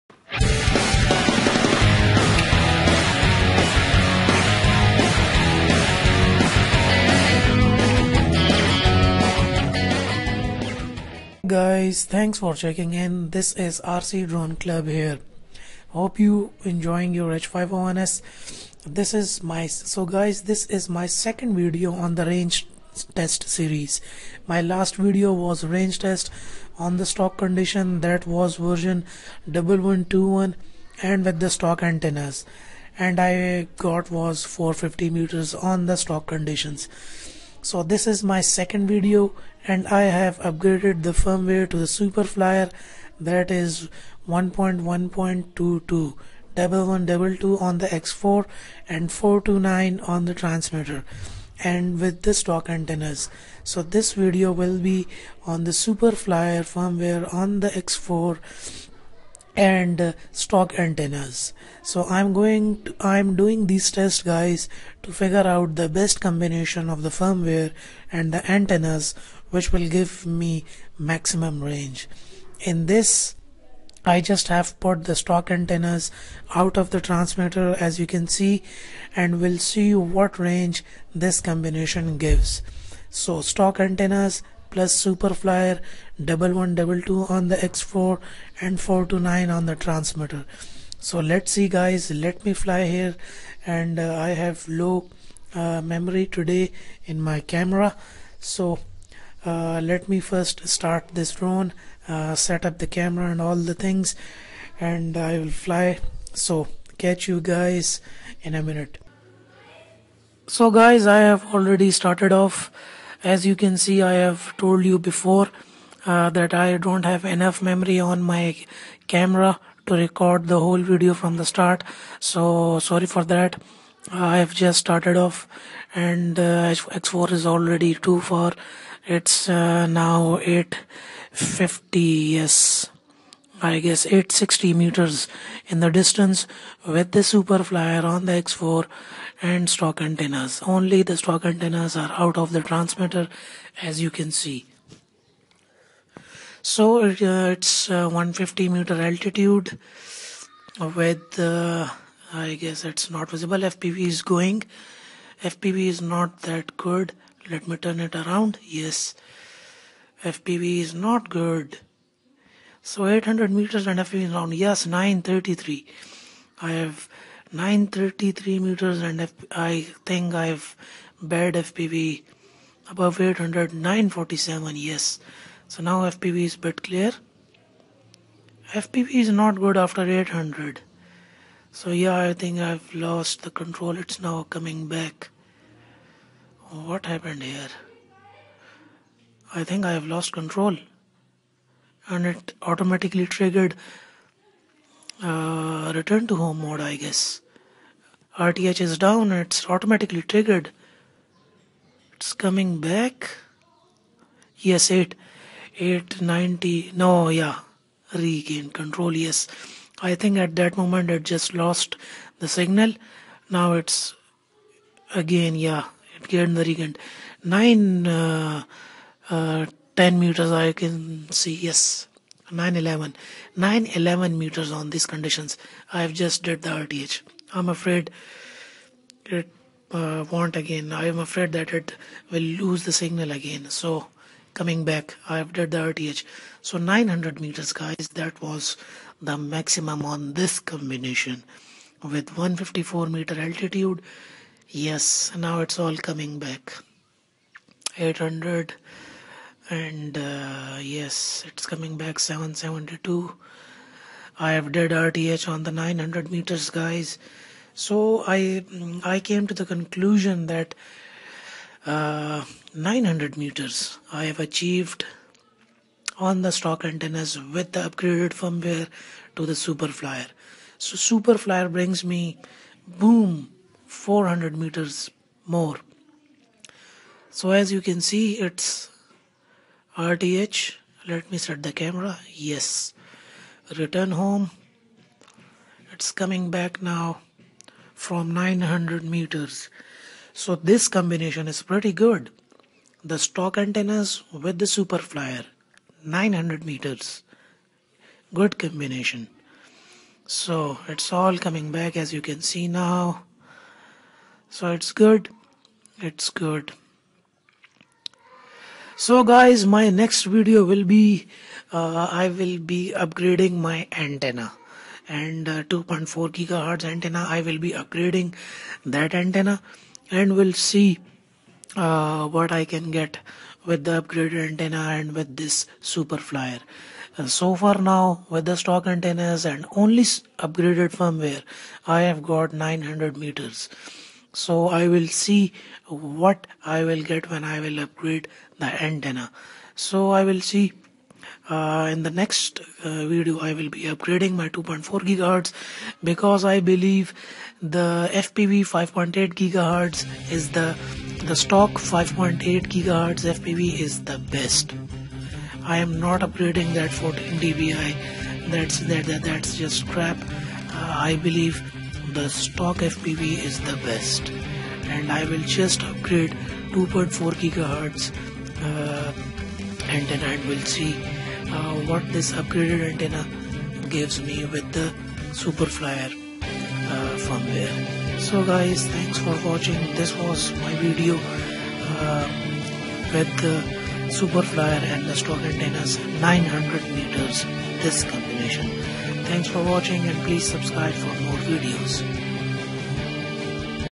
HUBSAN X4 H501S | RANGE TEST ON SUPER FLYER FIRMWARE
SO THIS IS THE 2ND VIDEO ON RANGE TEST SERIES

so guys here i am with my new range test video series with this hubsan h501s x4 fpv in these videos i will be testing the hubsan h501s ranges under different circumstances and with the different combination of the firmware and antennas and will check what is the best condition and combination of the antennas , firmware , and location , the city area and country side and in last will choose the best combination which will give maximum range

★☆★ RECOMMENDED HUBSAN VIDEOS: ★☆★